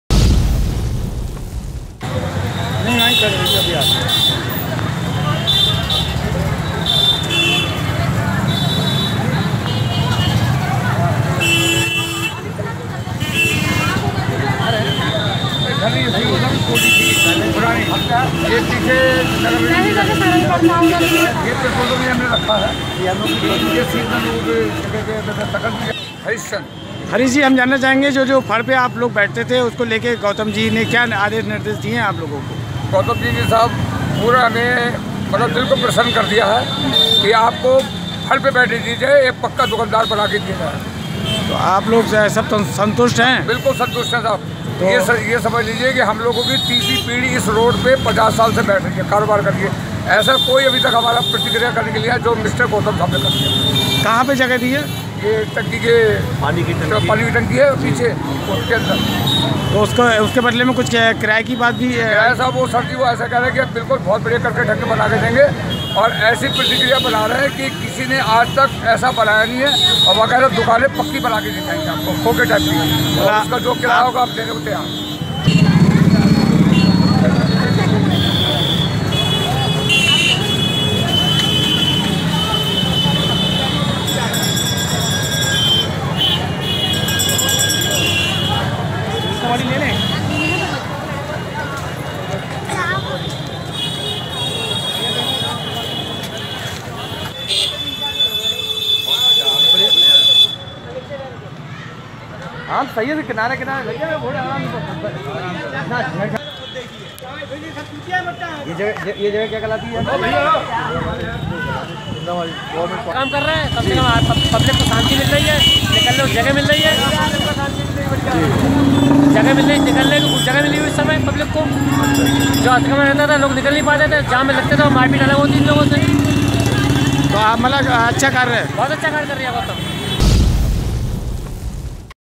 Bareilly News| फड़ व्यापरी को महापौर उमेश गौतम द्वारा दी गई शहर में जगह |ALL RIGHTS
बरेली में जिला स्तर पर लगने वाले फड़ व्यापरी को महापौर डा० उमेश गौतम द्वारा दी गई शहर में जगह